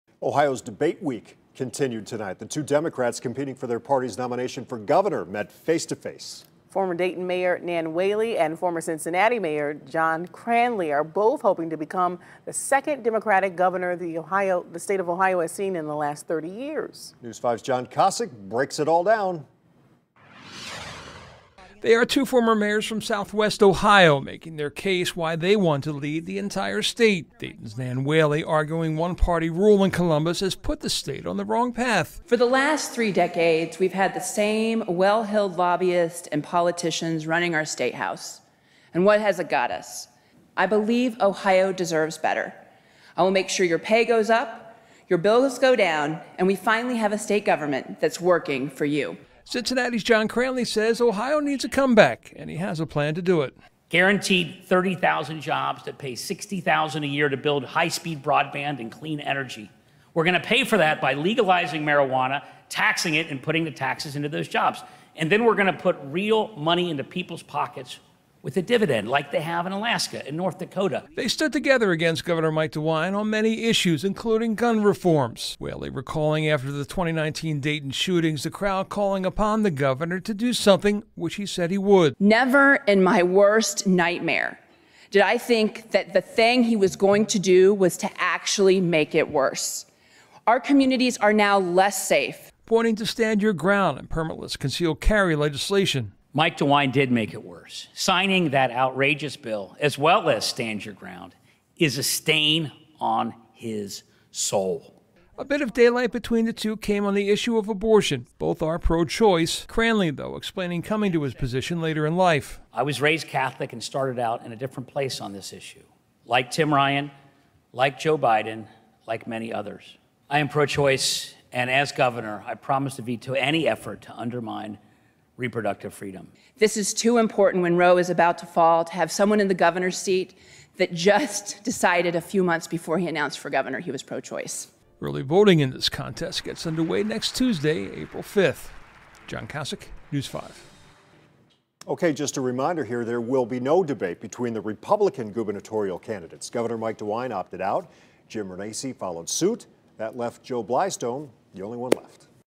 Democratic hopefuls for governor Nan Whaley, John Cranley go head to head
Democratic hopefuls for governor go head to head in debate